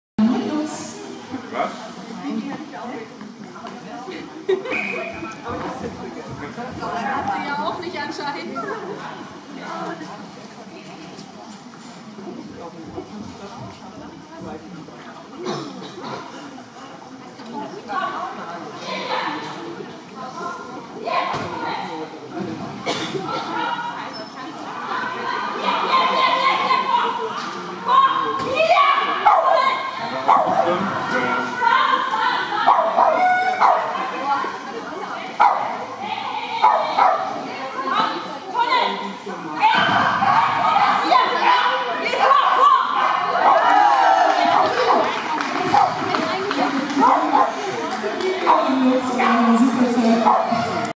Nicola und Lux, J2, 31.03.18
Nicola und Lux gewinnen den J2 in einer Zeit von 26:65